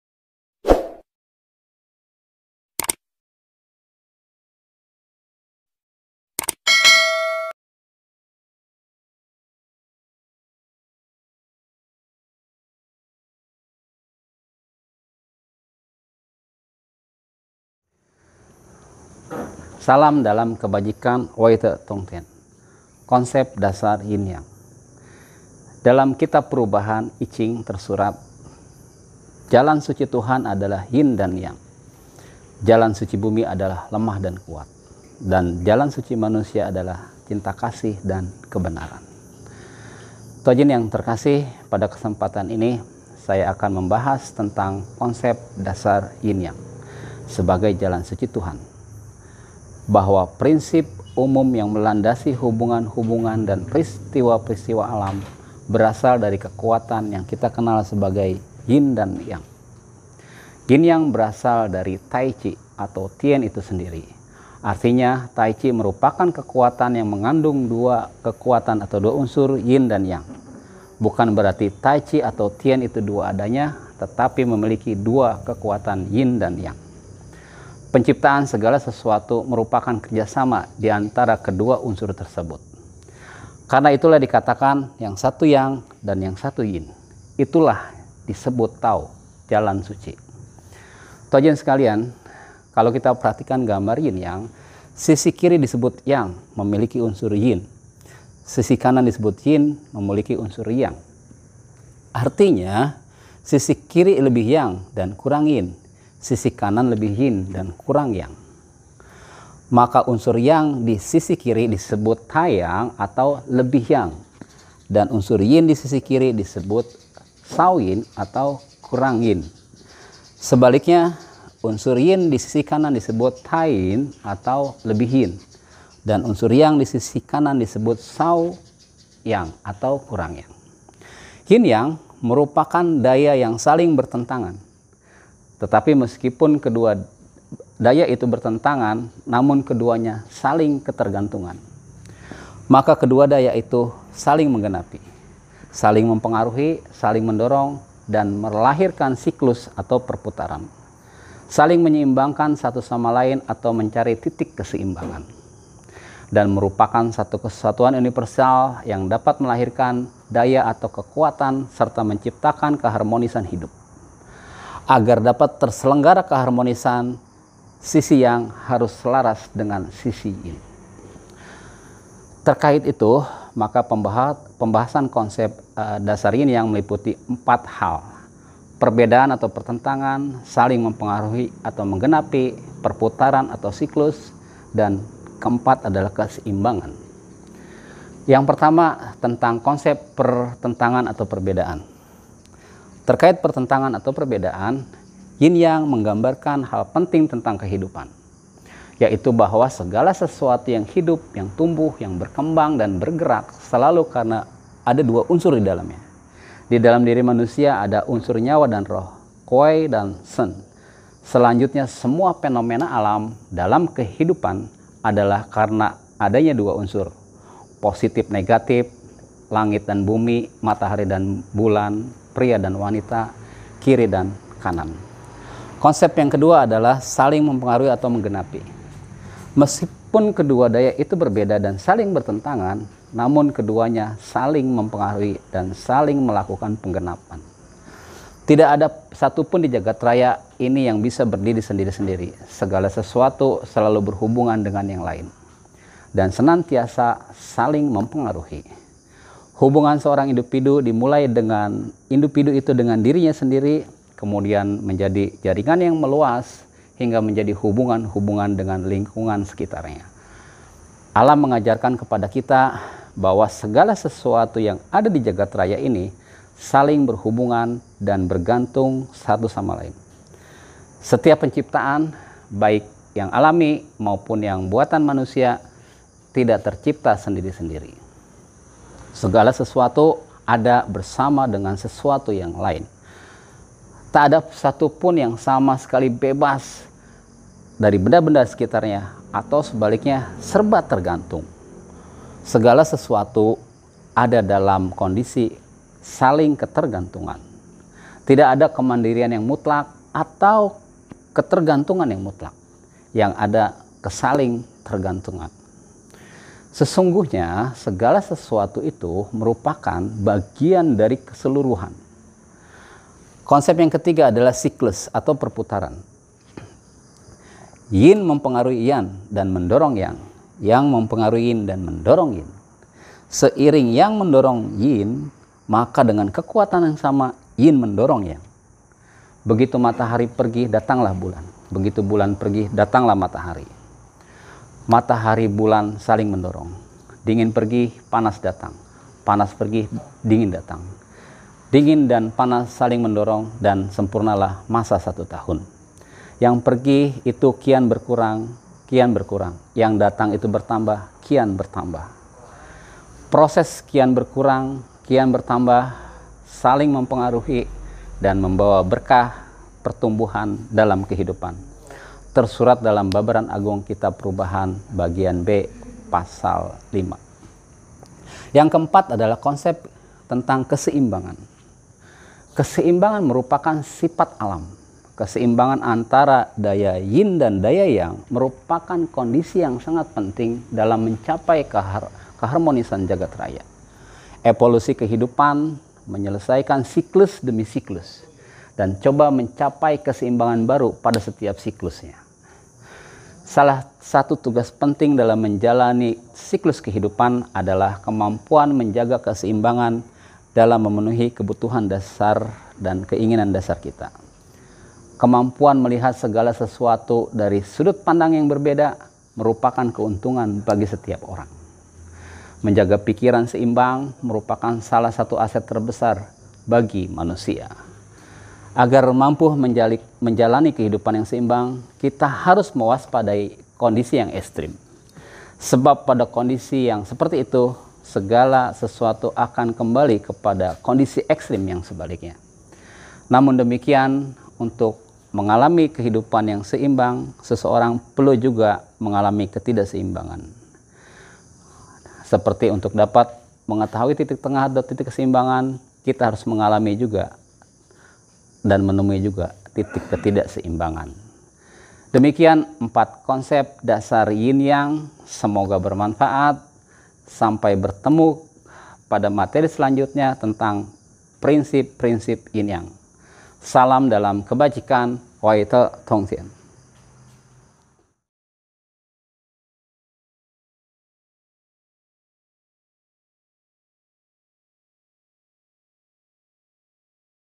KHONGHUCU "KONSEP DASAR YIN YANG."
Keseimbangan antara daya Yin dan daya Yang menjadi unsur sangat penting dalam mencapai keharmonisan jagat raya. evolusi kehidupan menyelesaikan siklus demi siklus, dan mencoba mencapai keseimbangan baru pada setiap siklusnya